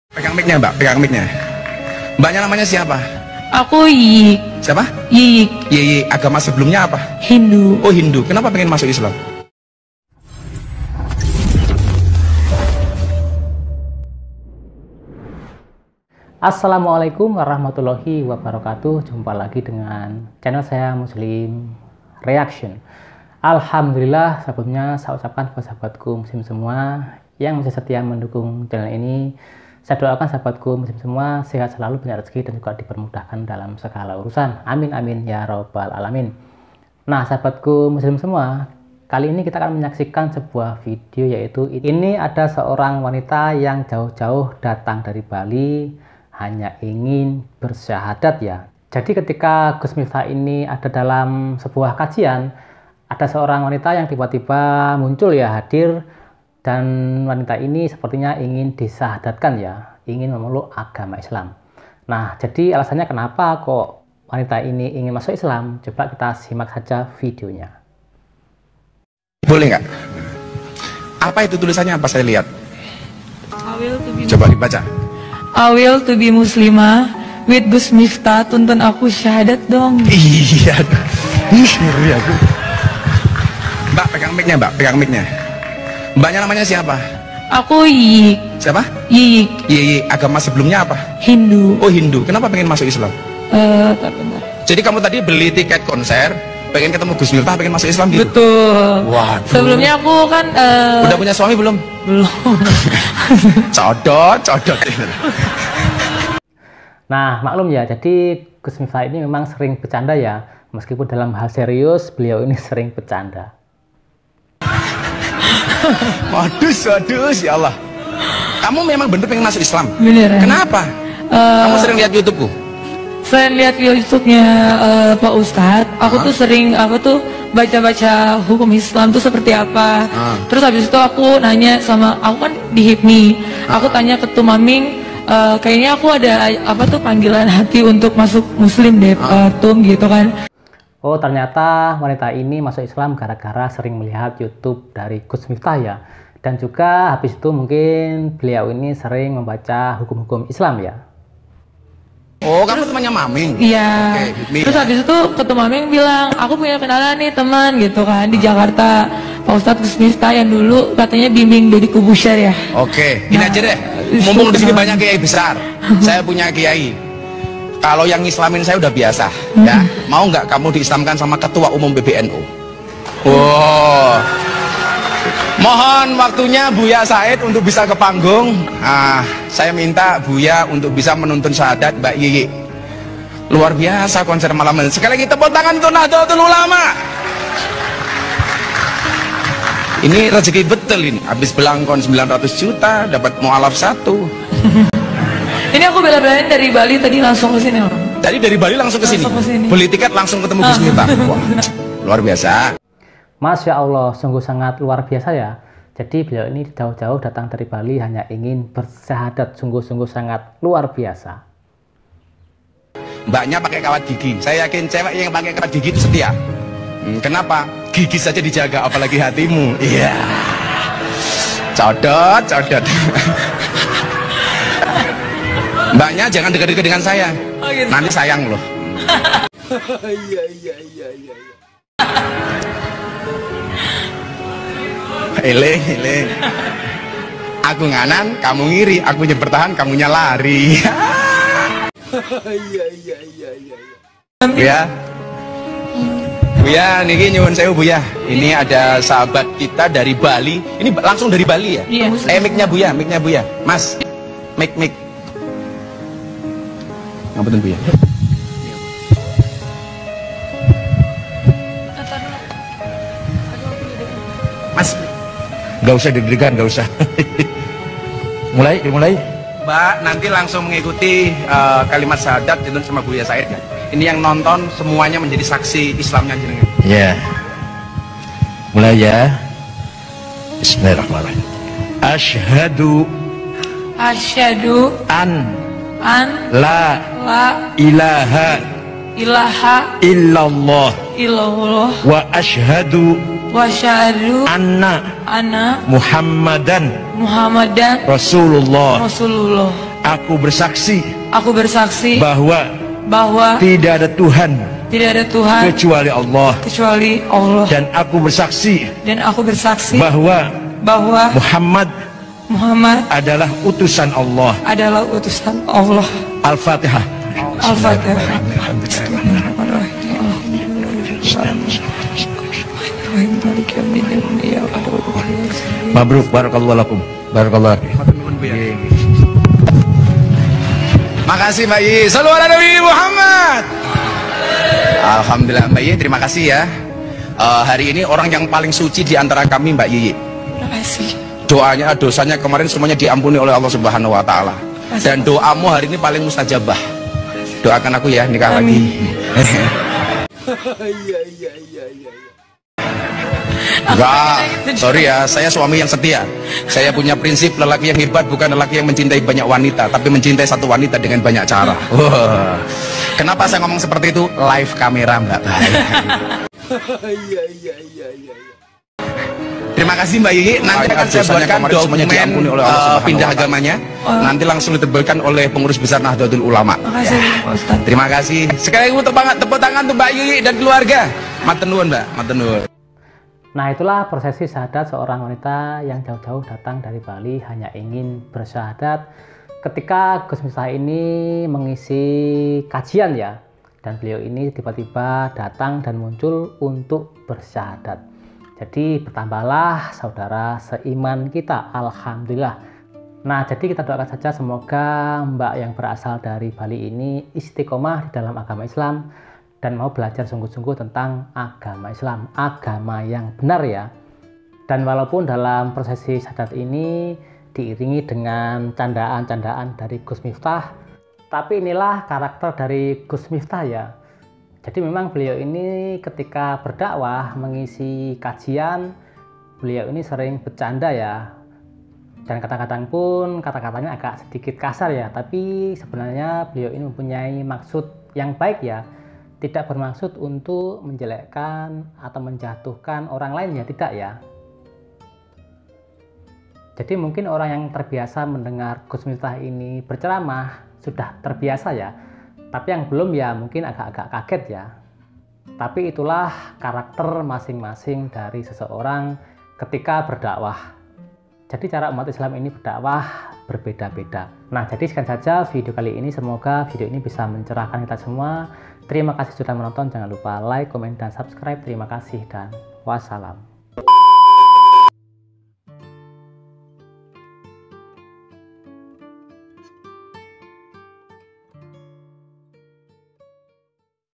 🔴karena ini... gadis hindu terbang dari bali ke jakarta ingin di syahadatkan gus miftah
gadis hindu bali
syahadat